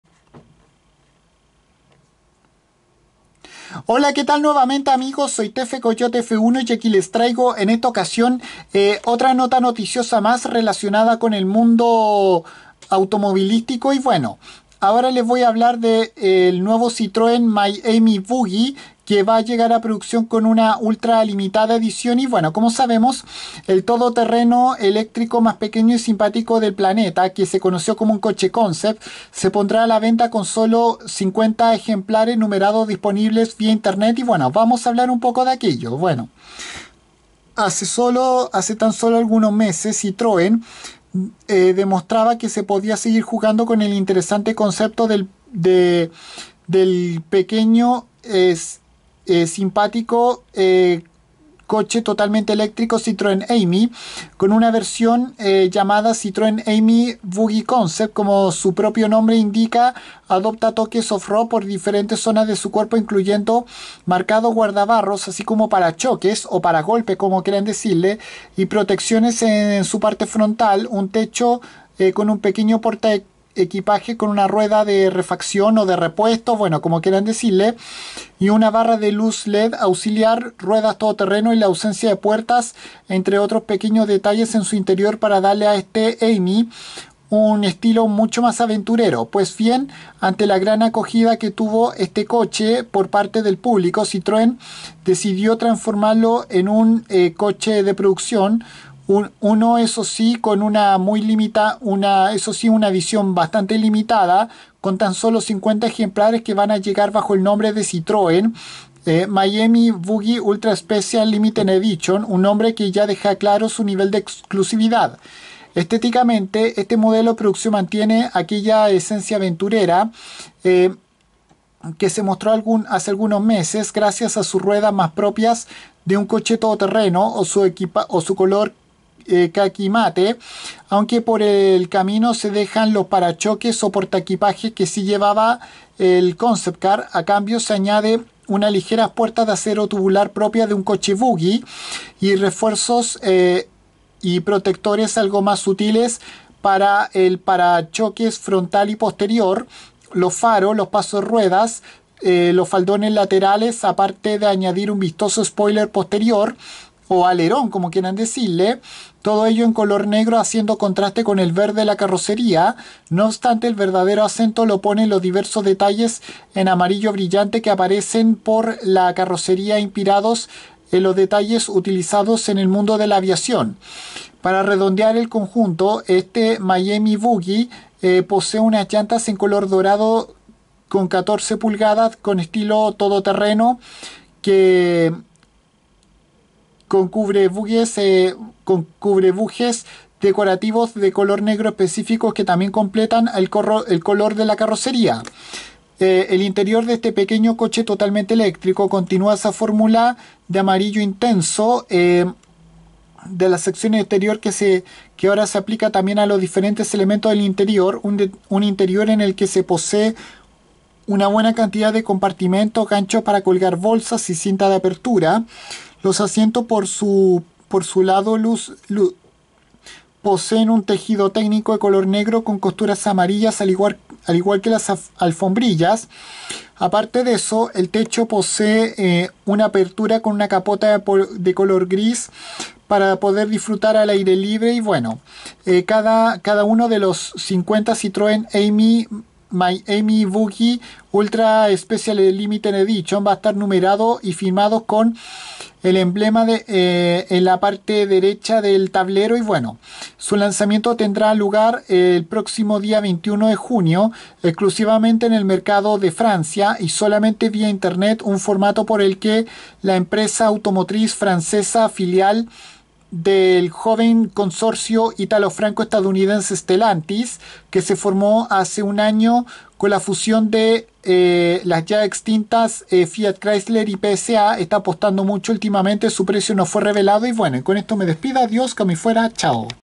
Citroen My Ami Buggy Concept información
En este vídeo les hablo sobre esta exclusiva y aventurera versión del simpático y compacto coche totalmente eléctrico de la firma automotriz francesa filial del joven consorcio automotriz ítalo-franco-estadounidense Stellantis NV que solo tendrá 50 ejemplares y cuya reserva solo podrá hacerse vía internet